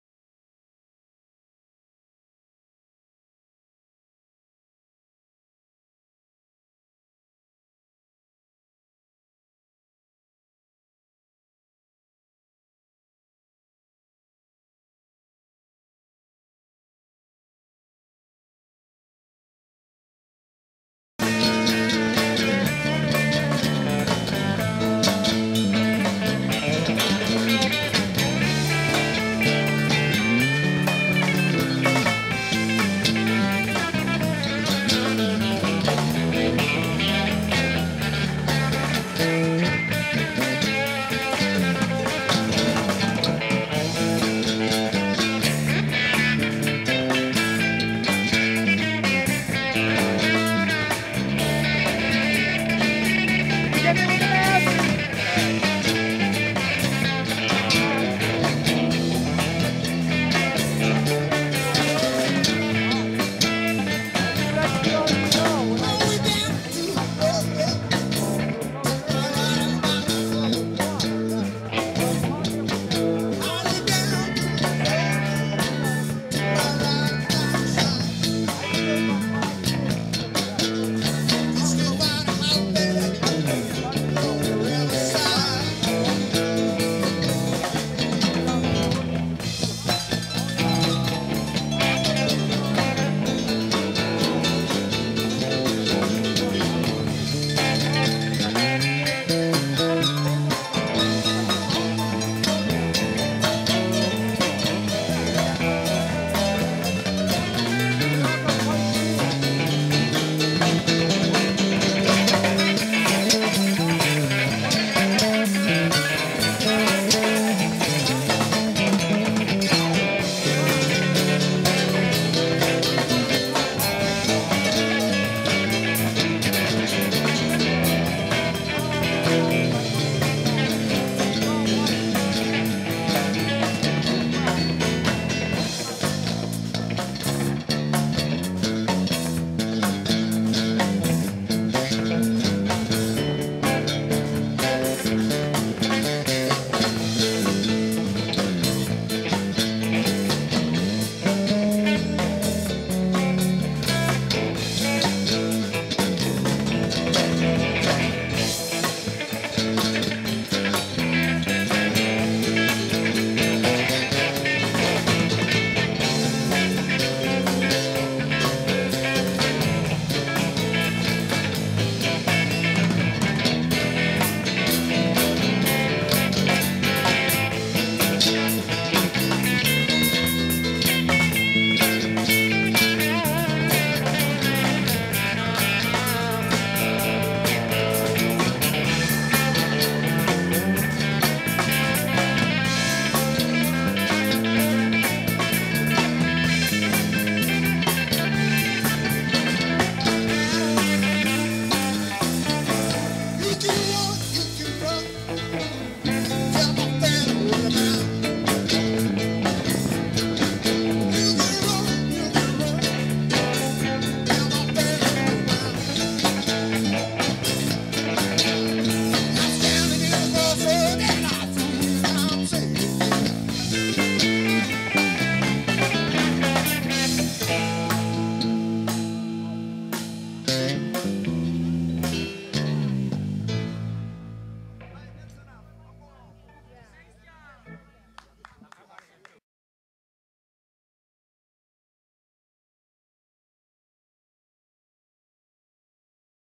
2020 SOCIAL DISTANCE LIVE SERIES #13 - CROSSROADS - TOM & FRIENDS (5/24/2020)
LIVE 2020 SOCIAL DISTANCE LIVE SOUNDS #13 ! ! ! !   CROSSROADS - TOM & FRIENDS ! ! ! !
RECORDED LIVE IN MIDDLEBORO, MA. ON SUNDAY MAY 24, 2020.